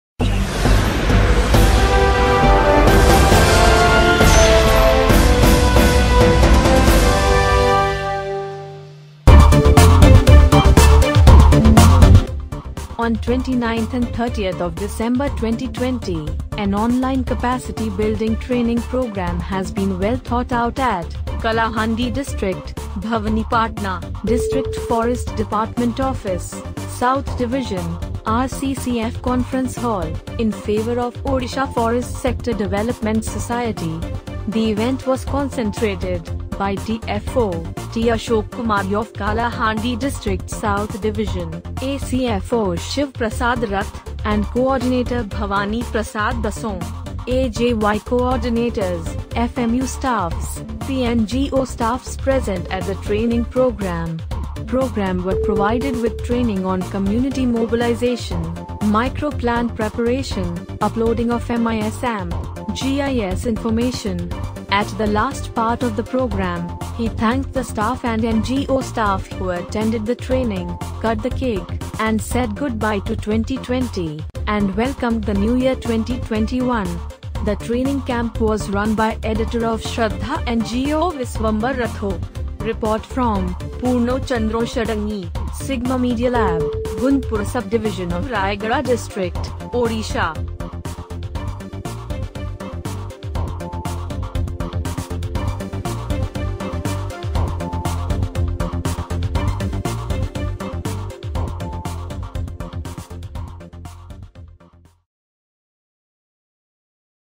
SIGMA BREAKING NEWS An Online Capacity Building Training program, Forest Dept., Kalahandi.
On 29th and 30th of December 2020, An Online Capacity Building Training program has been well thought-out at , Kalahandi District,  Bhawanipatna, District Forest Department Office, South Division, RCCF Conference Hall, in favor of Odisha Forest Sector Development Society. The event was concentrated by DFO T Ashok Kumar of Kalahandi District South Division, ACFO Shiv Prasad Rath, and Coordinator Bhavani Prasad Das. AJY coordinators, FMU staff, PNGO staff present at the training program were provided with training on community mobilization, micro-plan preparation, uploading of MIS / GIS information. At the last part of the program, he thanked the staff and NGO staff who attended the training, cut the cake and said goodbye to 2020, and welcomed the new year 2021. The training camp was run by editor of Shardha N G O Biswambar Rath